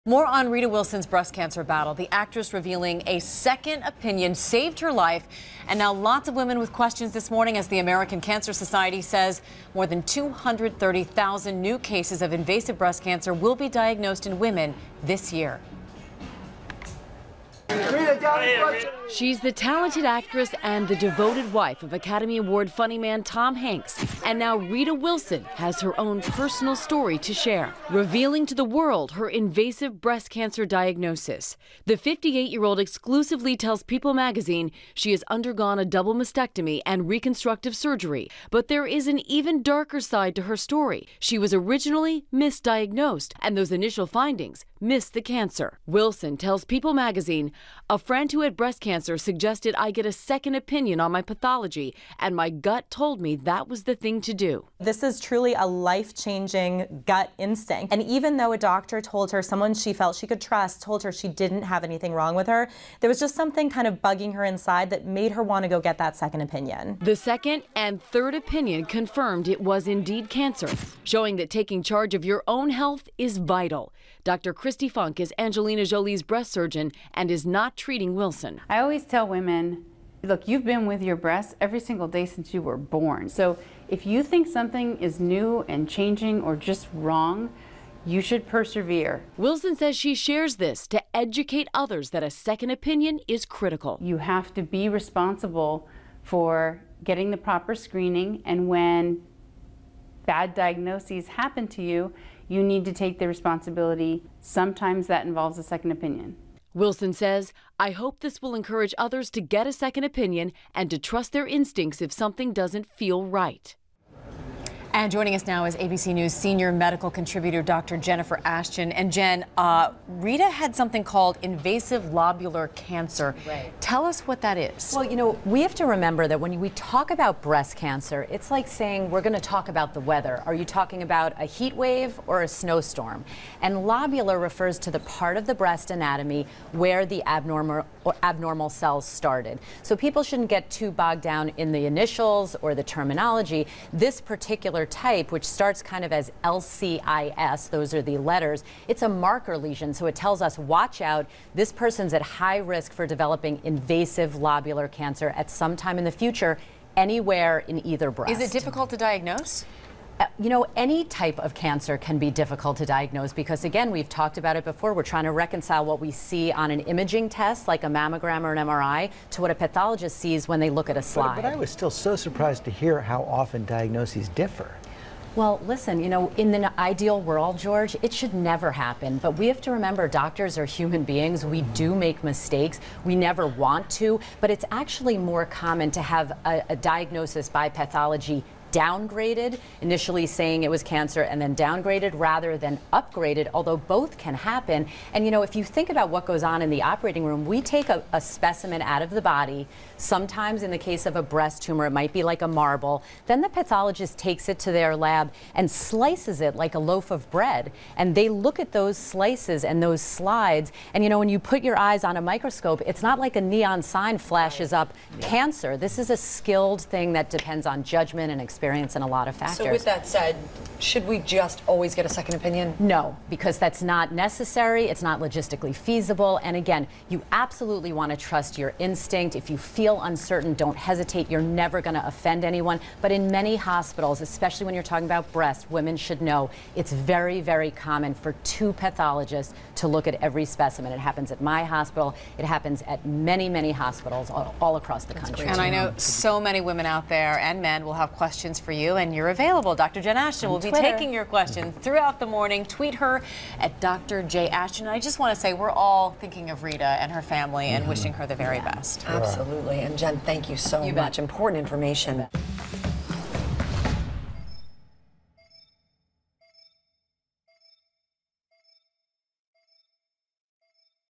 Rita Wilson’s Breast Cancer Diagnosed after Second Opinion
Wilson tells People magazine she was originally misdiagnosed, but decided to get a second opinion.